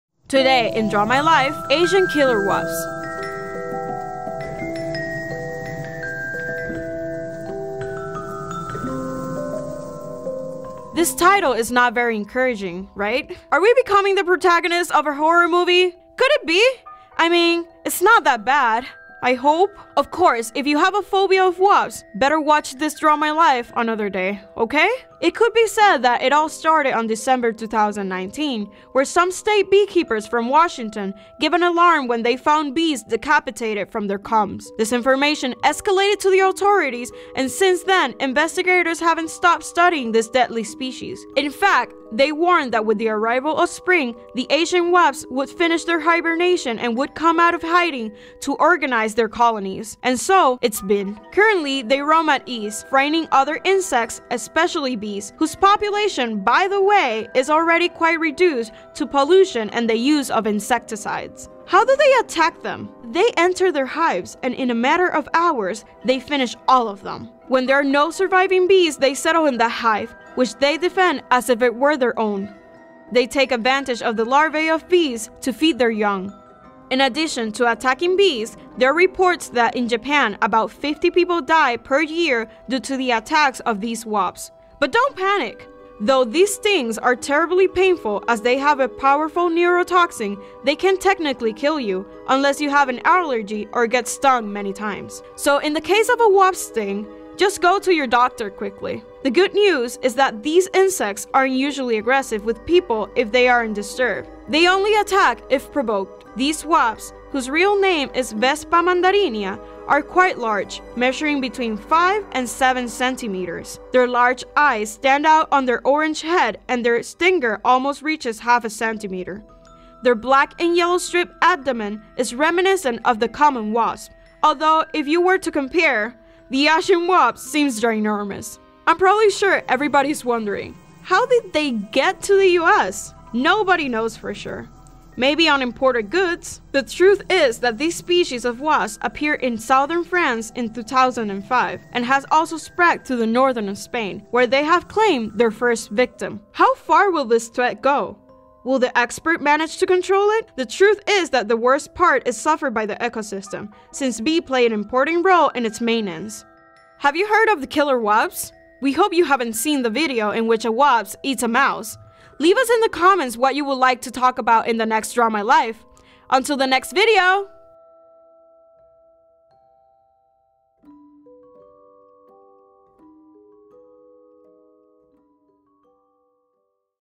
KILLER WASPS 🐝 | Draw My Life
Did you know that a species of Asian wasps can kill a human with a bite? Today in Draw My Life we tell you all the details of these dangerous insects known as "Vespa Mandarinia". Leave us in the comments what would you life to talk about in the next Draw My Life!